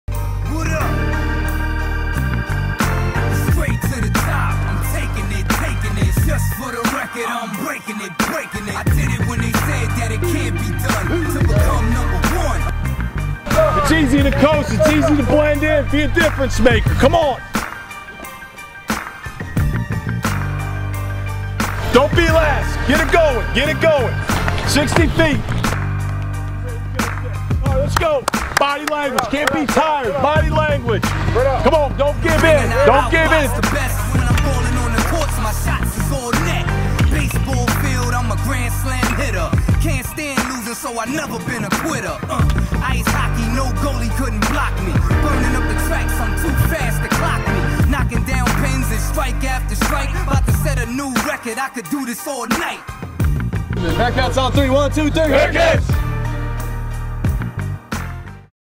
Cincinnati Bearcats 2017 Fall Baseball Sights and Sounds
Sights and sounds from 2017 fall ball for the Bearcats baseball team.